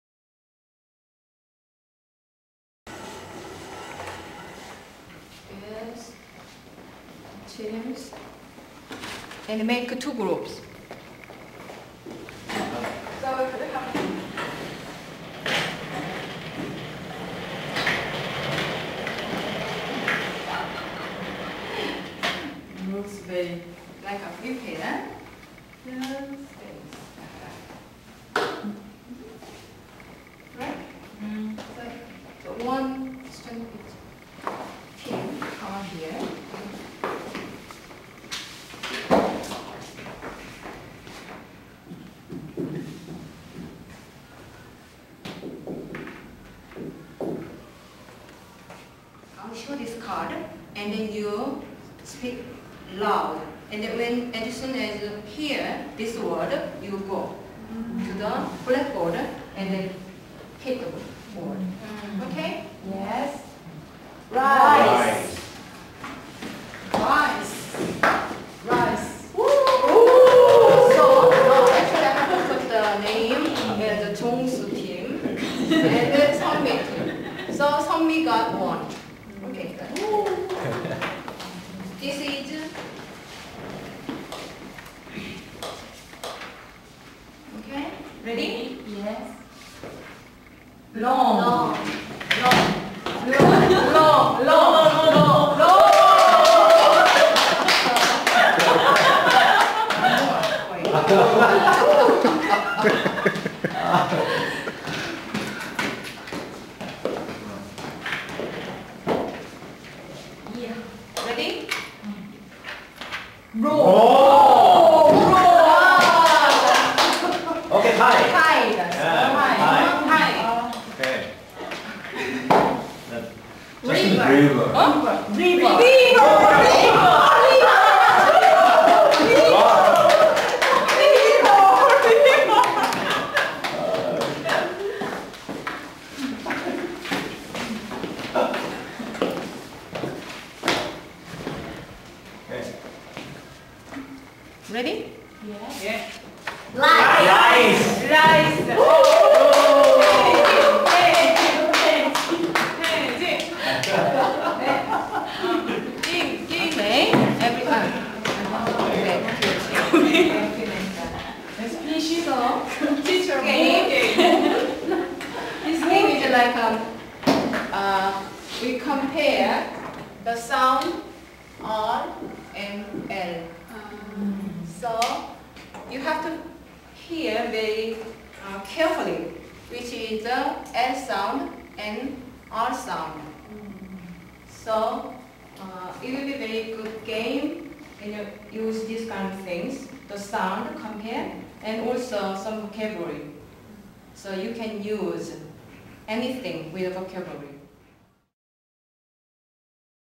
Fly Swatter by Lee Mi-sook
Name: Lee Mi-sook

Activity Title: Fly Swatter

Skills used: Listening and Reading

Materials: two fly swatters, written vocabulary words on a board (or pictures)

Preparation:
• Write vocabulary words scattered on a board (also pictures can be used).
• Place two chairs in front of the board with the fly swatters on a table.

Directions:
• Divide the class into two equal teams.
• Have one student from each team sit in the chairs with their backs to the board
• The teacher says one of the vocabulary words on the board
• Students stand up, grab a fly swatter and try to find the word on the board
• Students get one swat and when the first student hits the right word gets one point for the team
• Unless a student hits the right word, no point is awarded.
• The next student from each team is up.
• Repeat until everyone has had at least one turn.
• After the game, check the meaning of the words all together.

Considerations:
• This game can be used with any student level and any class size.
• You can add some misspelled words on the board to make the a